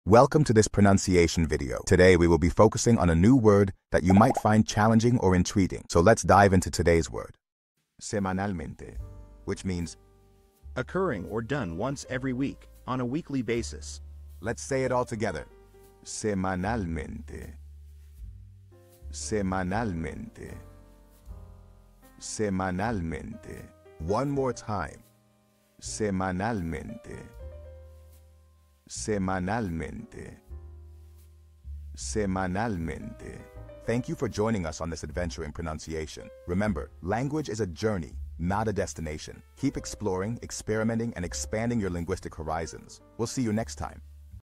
How to pronounce Semanalmente
Master the Pronunciation of 'Semanalmente Which Means weekly' - which means : Occurring or Done Once Every Week;on a weekly Basis. 📝 with @PronunciationSpanish 🎓🗣️ - [Your Guide to Spanish]

Learn to pronounce 'Semanalmente' perfectly in Spanish with our easy and engaging tutorial by @PronunciationSpanish! 📖📽️ In this comprehensive video, we provide you with native speaker examples 🗣️👂 and phonetic breakdowns 🎵 to ensure you get the pronunciation just right.

How to say Weekly in Spanish